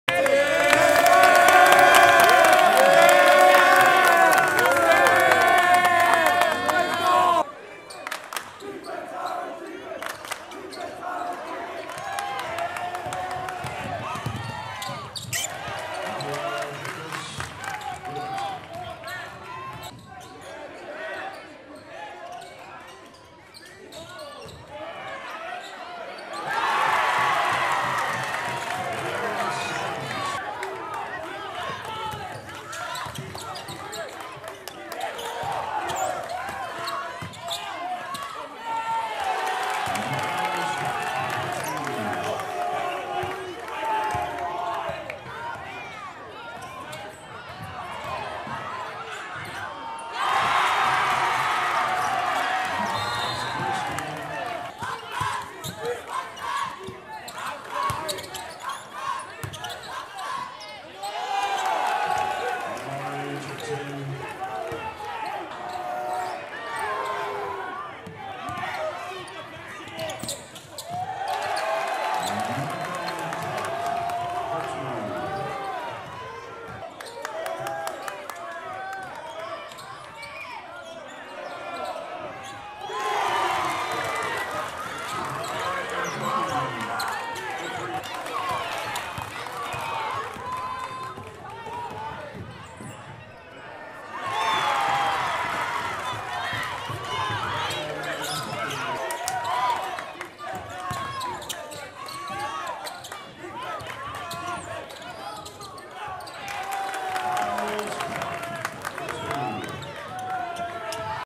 HS Basketball: Rainier Beach vs. O'Dea
Highlights of O'Dea's 68-51 win over Rainier Beach